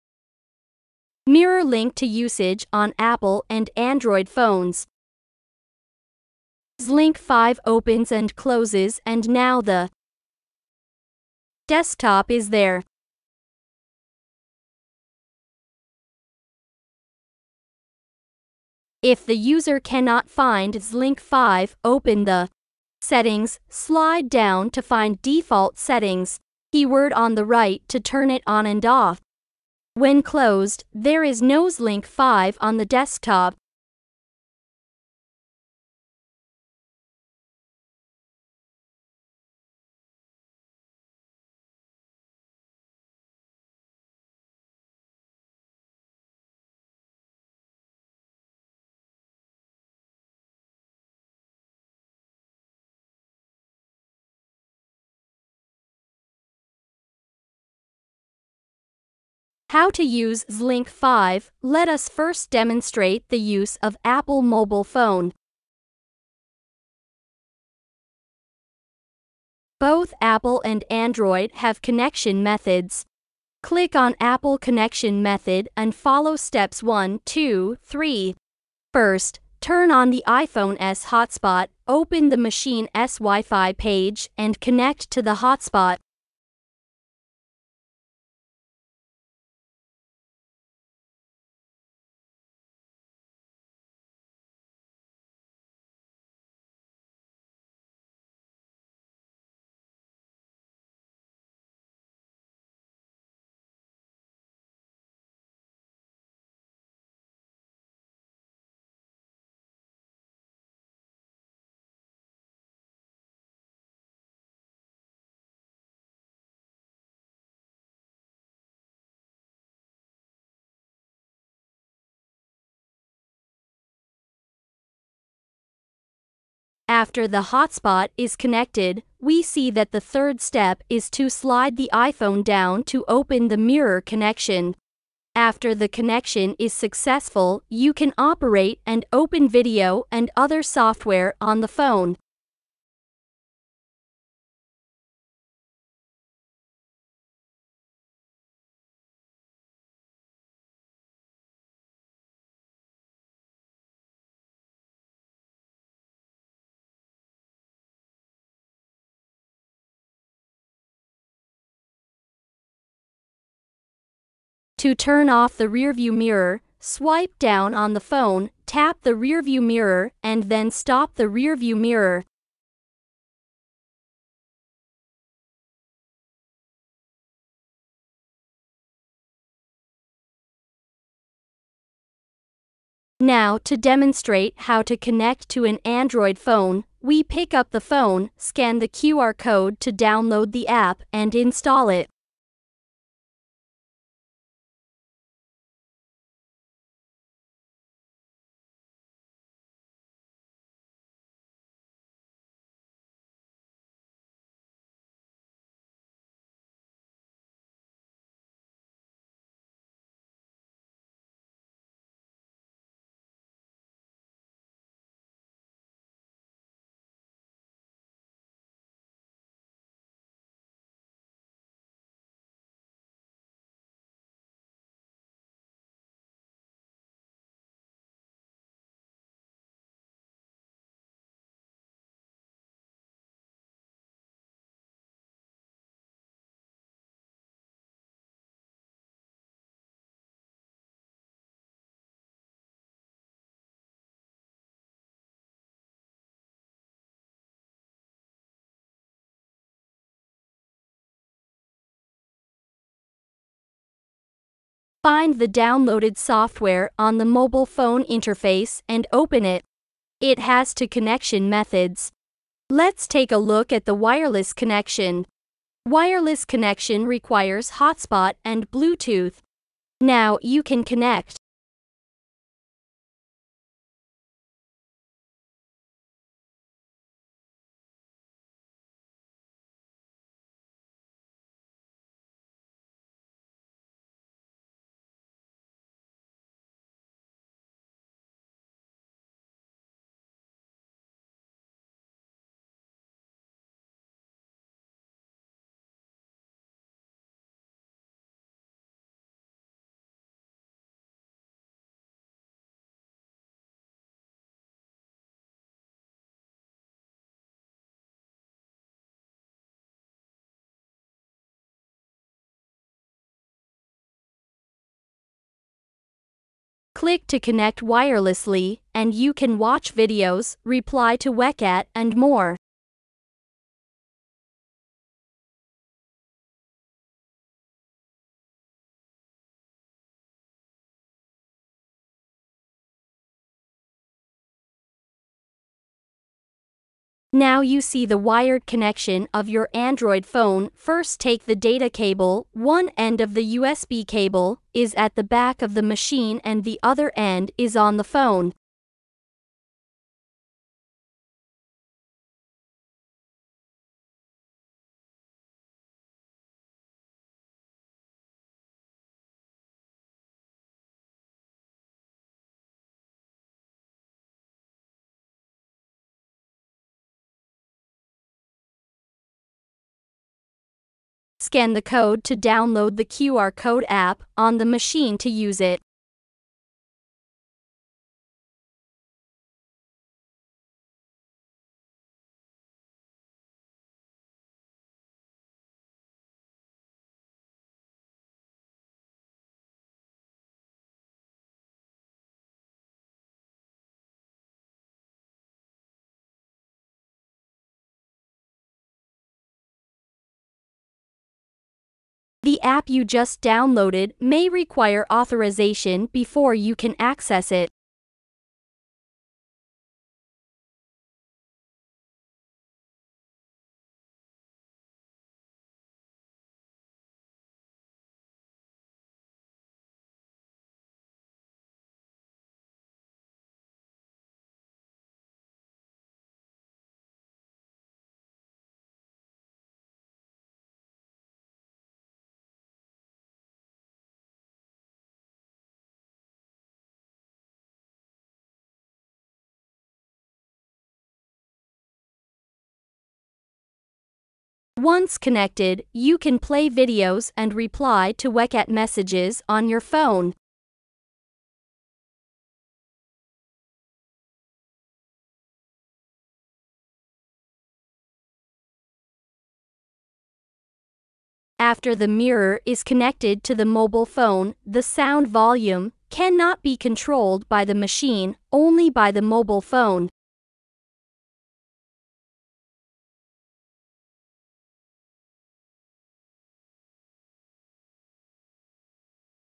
Car Android system player mirror link Apple phone screen mirroring Android phone screen mirroring
Mirror link to usage on Apple and Android phones
"ZLINK5" opens and closes and now the desktop is there
If the user cannot find "ZLINK5", open the settings, slide down to find "Default Settings", and click the word on the right to turn it on and off
When closed, there is no "ZLINK5" on the desktop
How to use "ZLINK5", let us first demonstrate the use of Apple mobile phone
Both Apple and Android have connection methods. Click on Apple connection method and follow steps 1, 2, 3. First, turn on the iPhone’s hotspot, open the machine’s WIFI page and connect to the hotspot
After the hotspot is connected, we see that the third step is to slide the iPhone down to open the mirror connection. After the connection is successful, you can operate and open video and other software on the phone.
To turn off the rearview mirror, swipe down on the phone, tap the rearview mirror, and then stop the rearview mirror
Find the downloaded software on the mobile phone interface and open it. It has two connection methods. Let's take a look at the wireless connection. Wireless connection requires hotspot and Bluetooth. Now you can connect.
Click to connect wirelessly and you can watch videos, reply to WeChat and more
Now you see the wired connection of your Android phone, first take the data cable, one end of the USB cable is at the back of the machine and the other end is on the phone
Scan the code to download the QR code APP on the machine to use it
The APP you just downloaded may require authorization before you can access it.
Once connected, you can play videos and reply to WeChat messages on your phone
After the mirror is connected to the mobile phone, the sound volume cannot be controlled by the machine, only by the mobile phone.